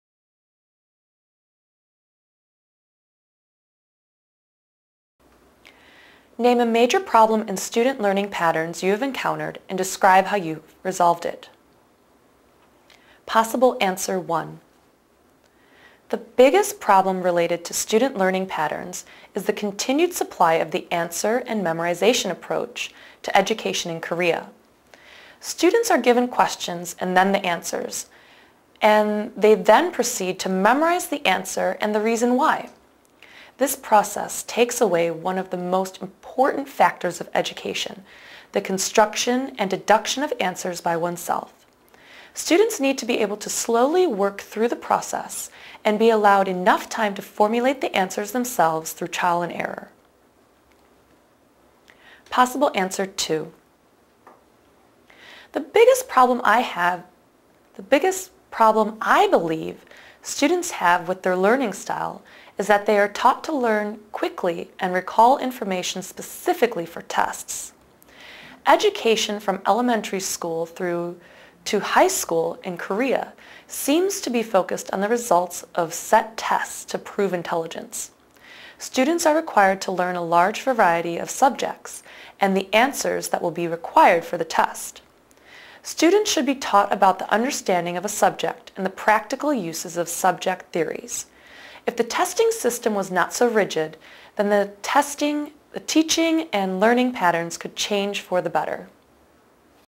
05_05 Name a major problem in student learning patterns
chap 05 05 Name a major problem in student learning patterns you have encountered and describe how you resolved it
101가지 질문으로 끝내는 교사임용 영어면접
Teacher Interview 101